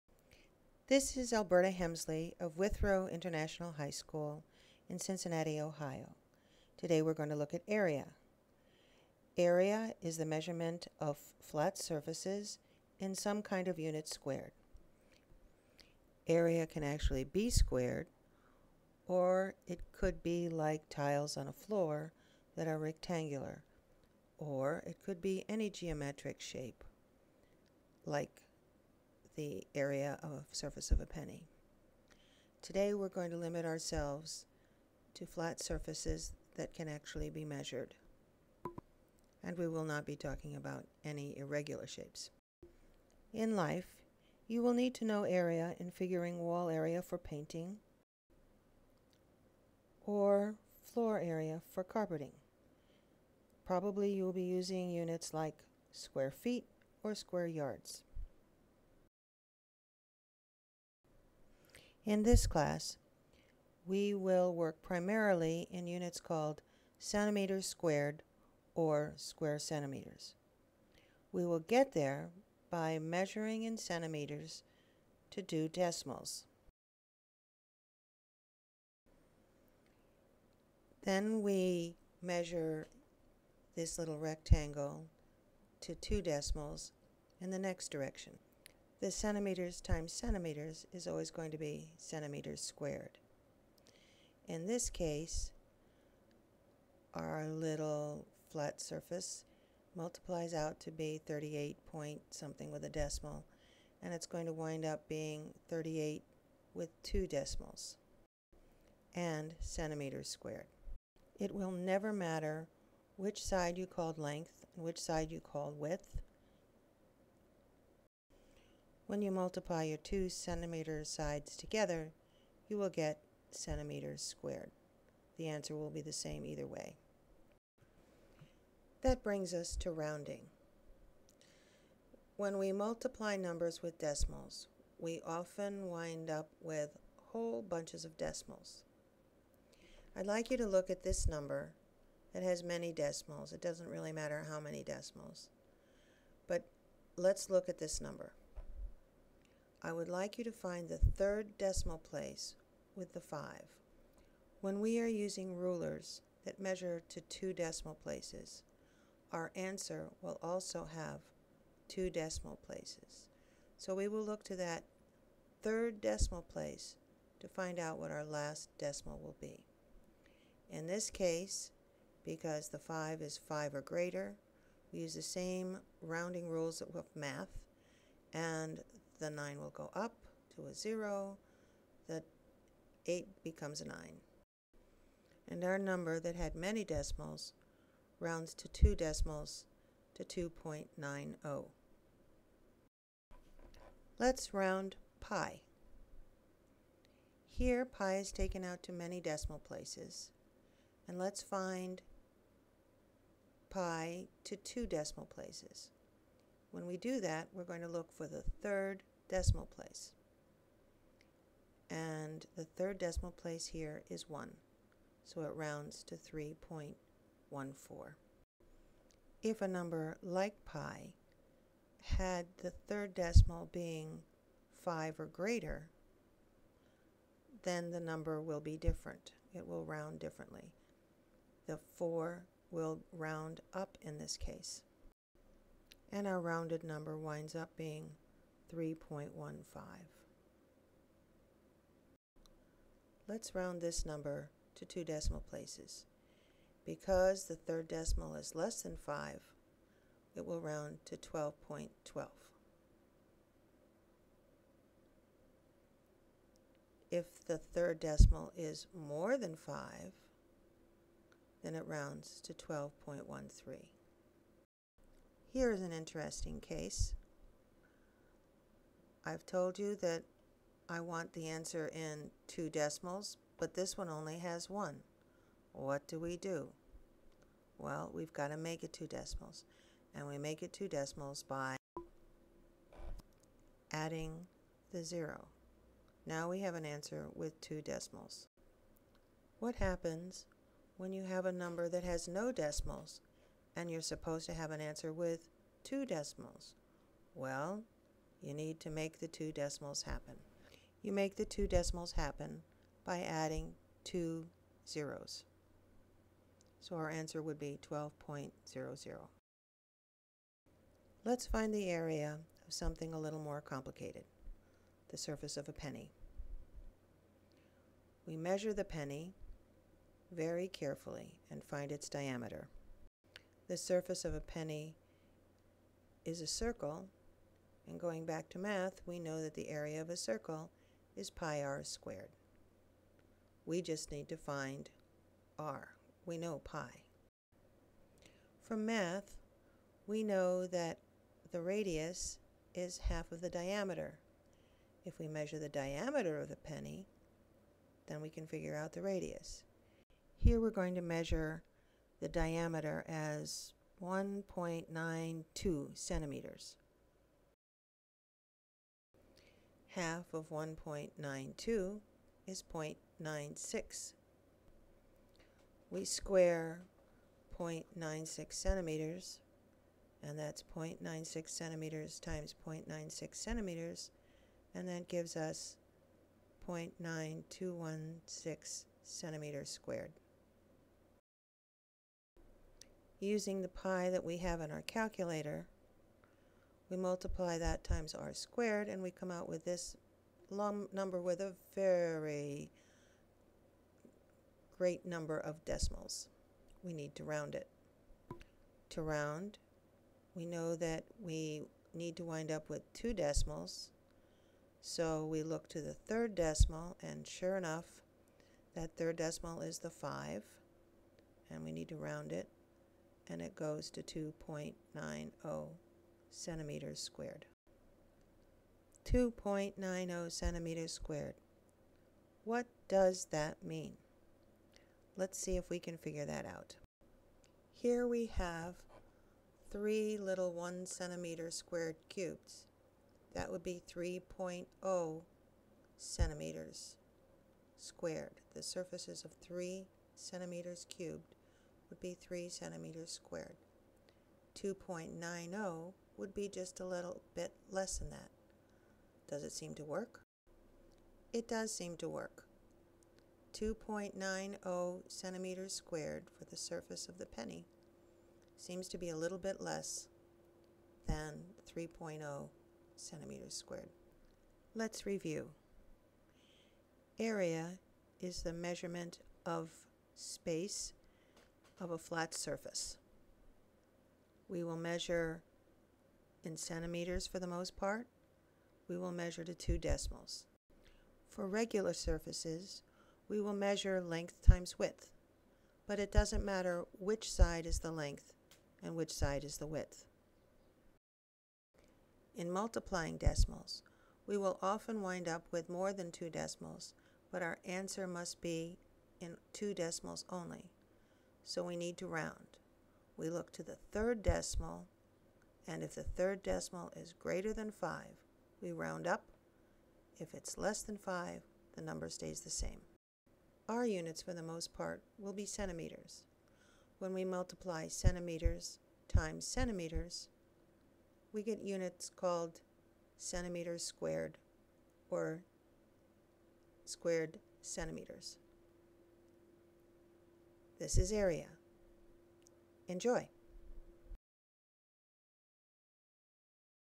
Area
Area of regular flat spaces is found by using a ruler measuring to 2 decimal places.   It does not matter which is length and which is width, the answer will be the same in centimeters squared or square centimeters.  The area of a penny is found.

Rounding follows the same rules as math.  Go to the decimal place one beyond the desired number of places and round up if the number is greater than 5.  The number stays the same if the next number is less than 5.